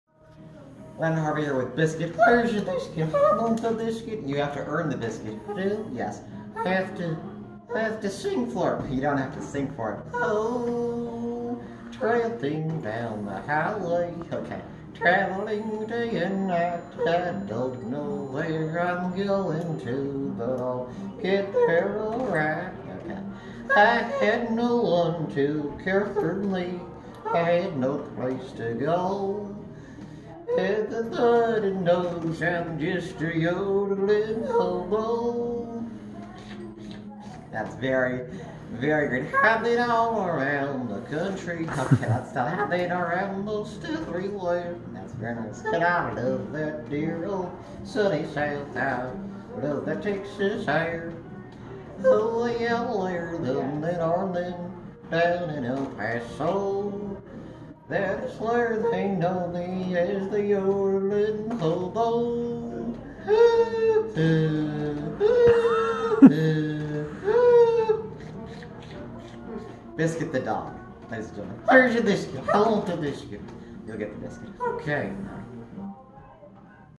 My Dog can Sing!?
My dog, Biscuit, tries to sing an old song for a treat...does he deserve one?